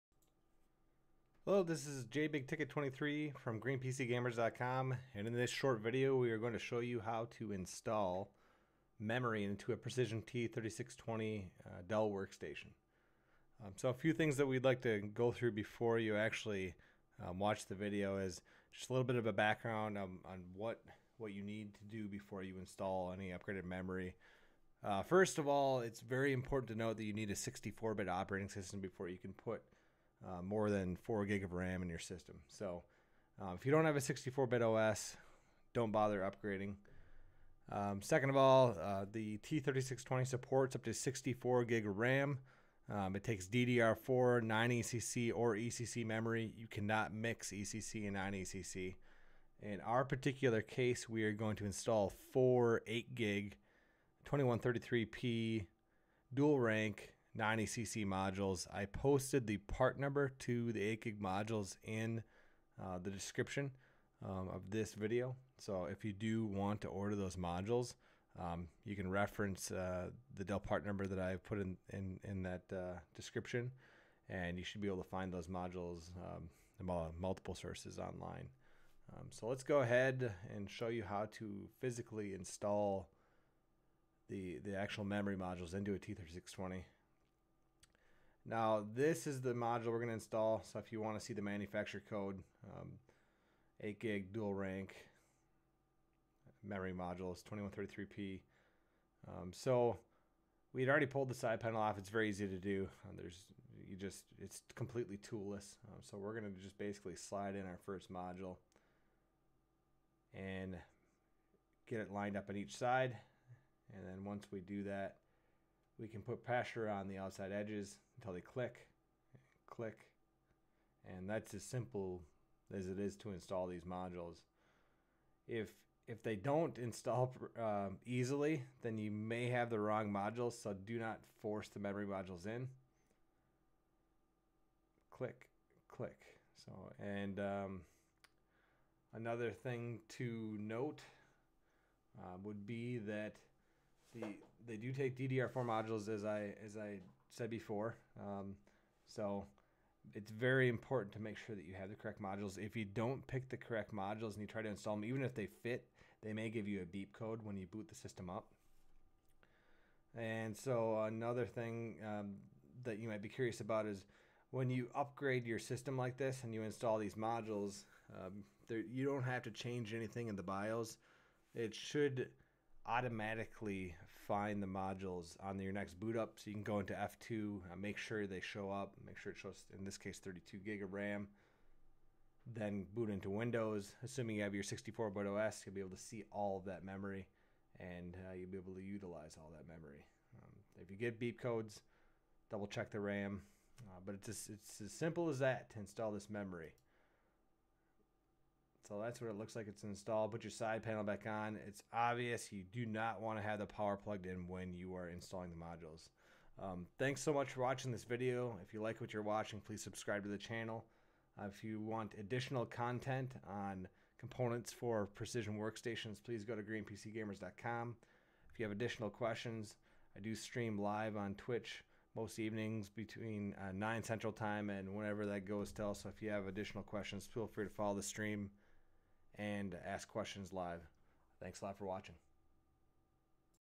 How to access and install memory in to a Dell Precision T3620 Workstation
This video will show you how to install memory in to a Dell Precision T3620 Workstation.

Memory Installed:

Dell FN6XK 8GB PC2133P DDR4 2RX8 Non-ECC (4 Modules)